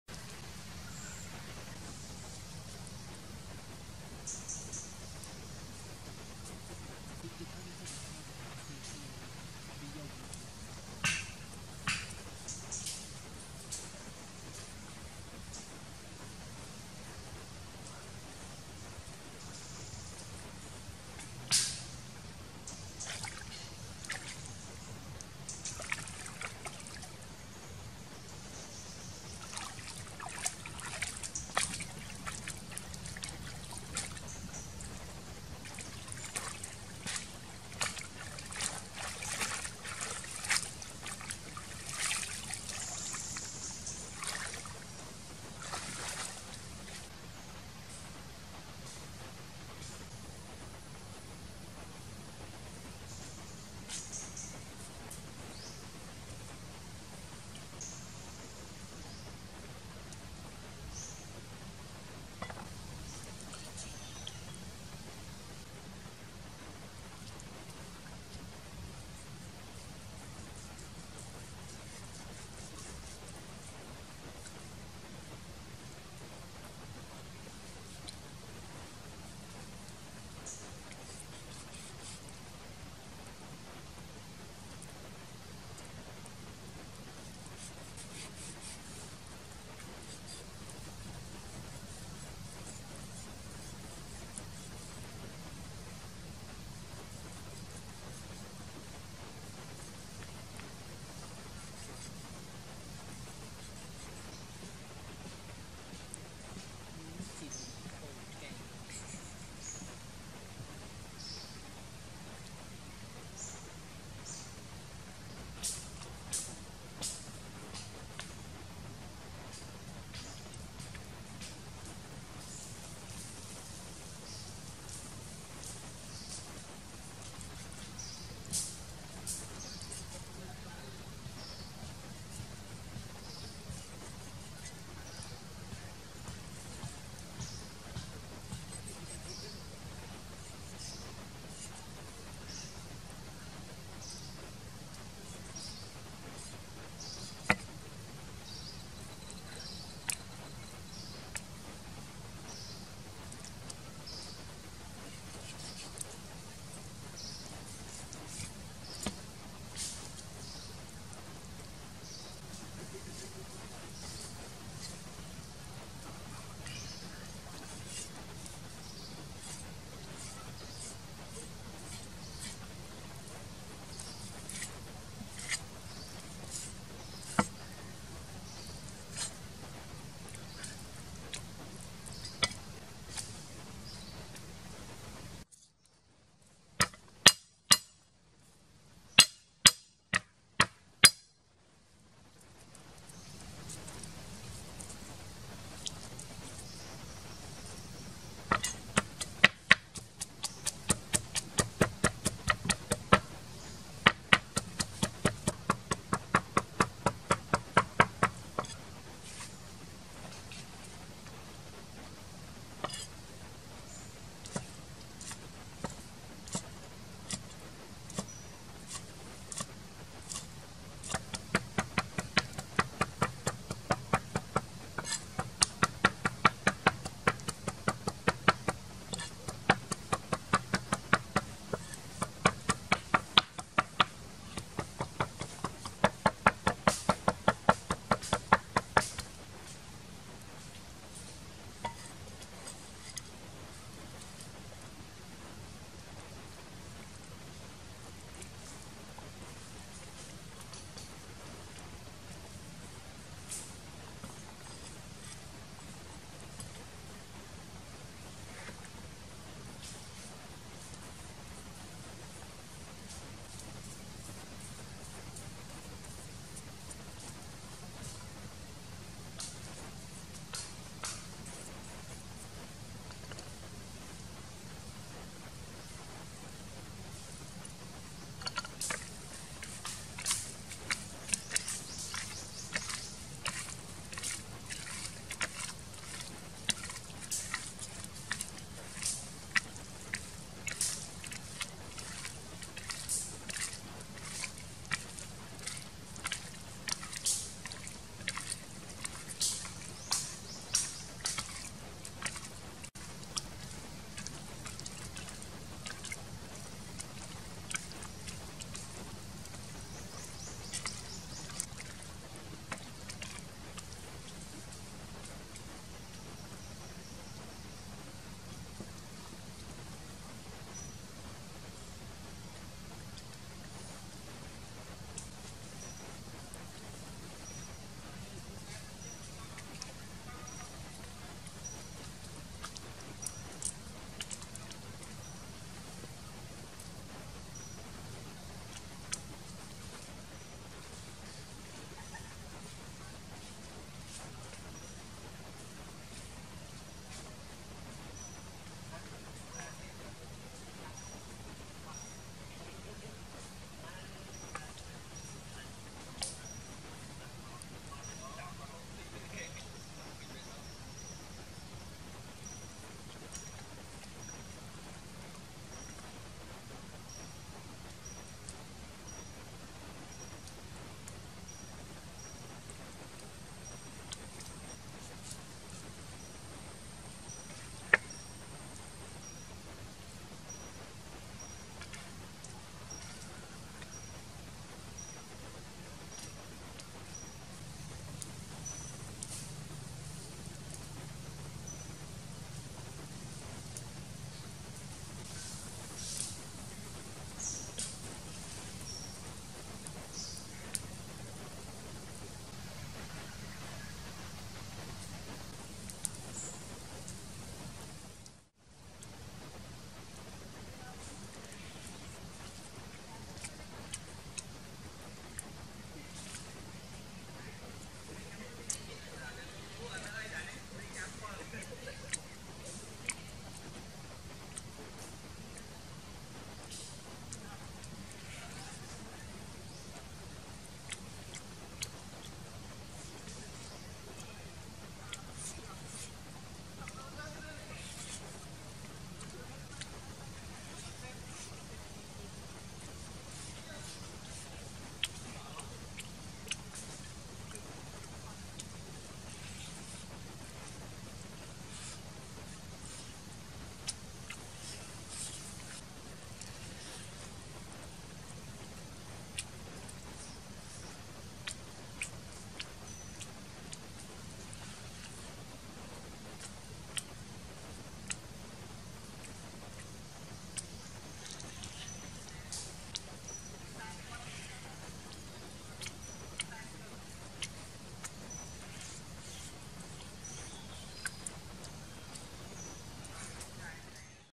Grilling Pig Heart And Pork Kidney Recipe, Pig Heart Special Ingredients Cooking Near The River
👉Turn on "cc" (subtitles) to display ingredients in your own language

Hi, this video I want to show you about: Grilling Pig Heart And Pork Kidney Recipe, Pig Heart Special Ingredients Cooking Near The River